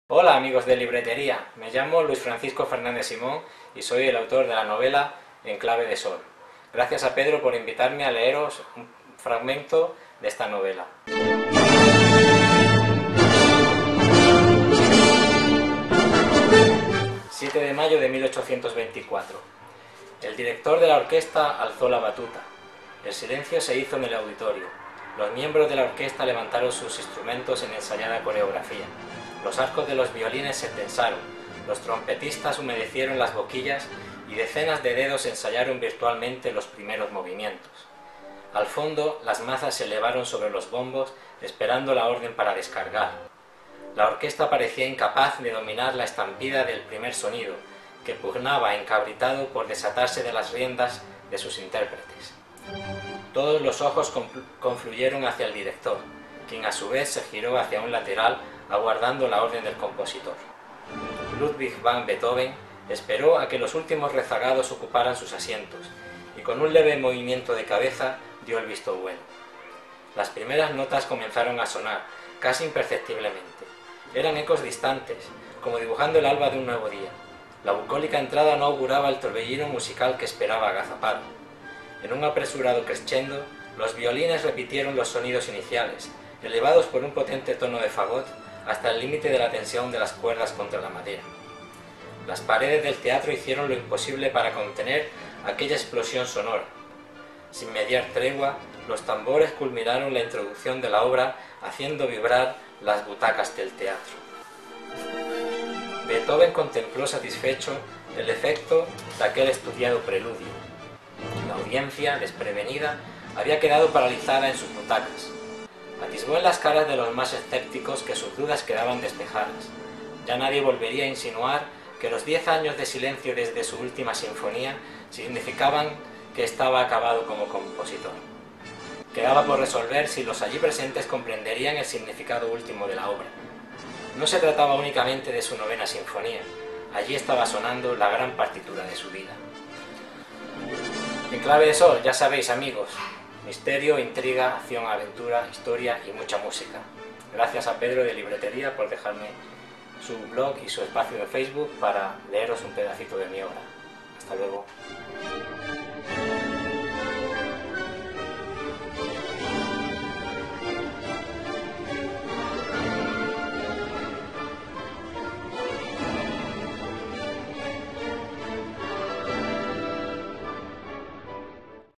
VídeoLectura: 'En clave de Sol', de Luis F. F. Simón
'En clave de Sol' es la primera novela del escritor granadino Luis F. F. Simón que nos presenta y lee un fragmento en este vídeo. Recoge el mito pitagórico de la música de las Esferas y traslada su búsqueda a los tiempos actuales. Conviven en la novela un dj, una abogada y un 'neuromúsico' con Pitágoras, Mozart y los masones de la Rosacruz. Hasta Beethoven se ve implicado en la búsqueda de una armonía que unirá a los hombres y mujeres de la tierra: